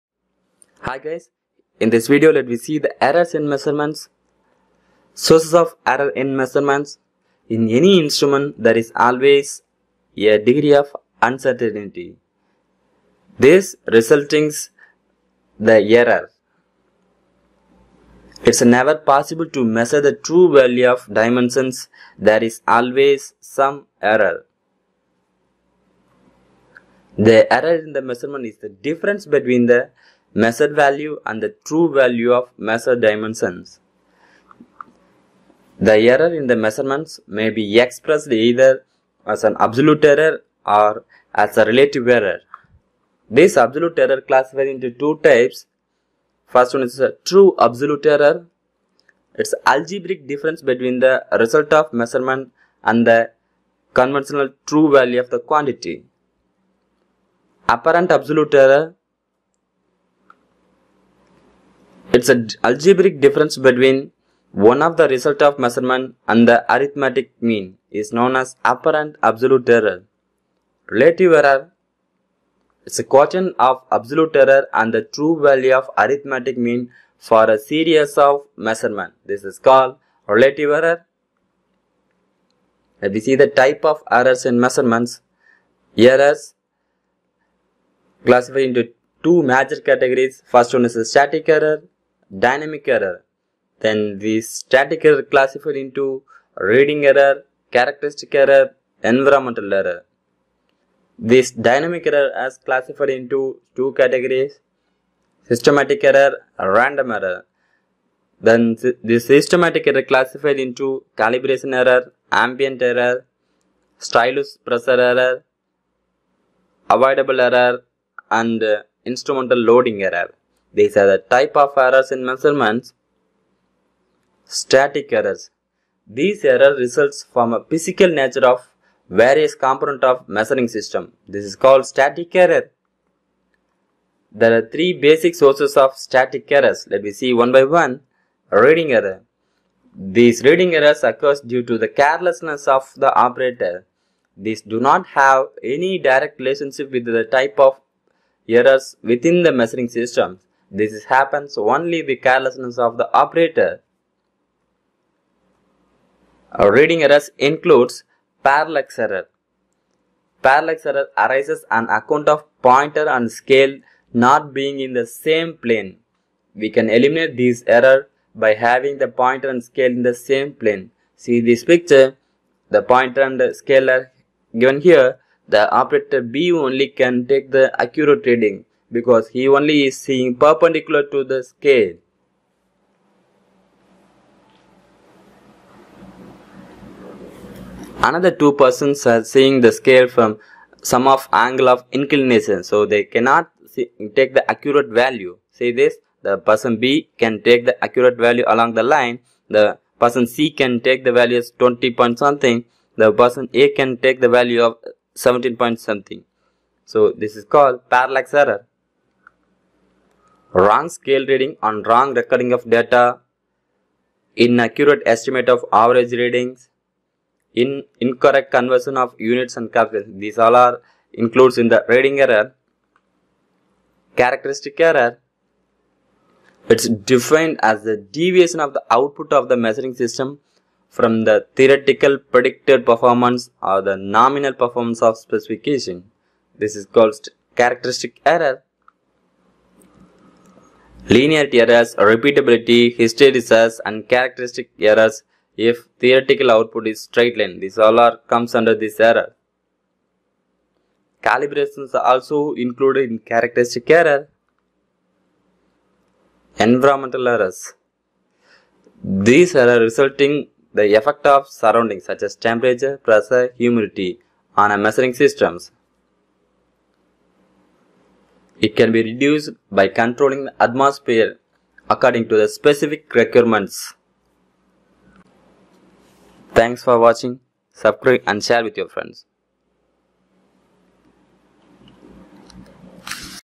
STATIC ERROR | ERRORS IN MEASUREMENT | TYPES OF ERROR | BEST ENGINEER
This channel is formed by faculty from BIT to enhance the knowledge of students towards technical and fundamentals
In this video we can learn classification of all types of error  static error and its detail.